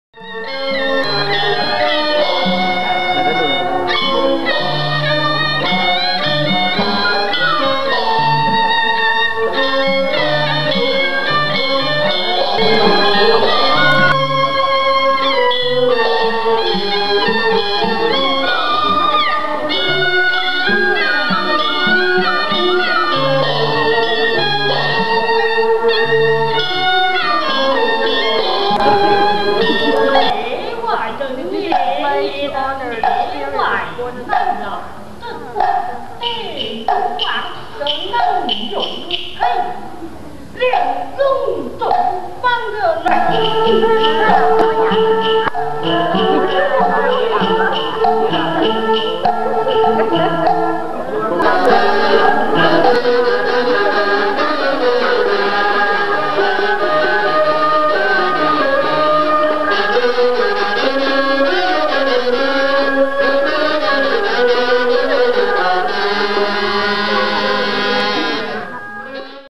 Jingxi Museum Performance
A clip from a Zhuang music performance at the Jingxi Zhuang Museum, May 2004.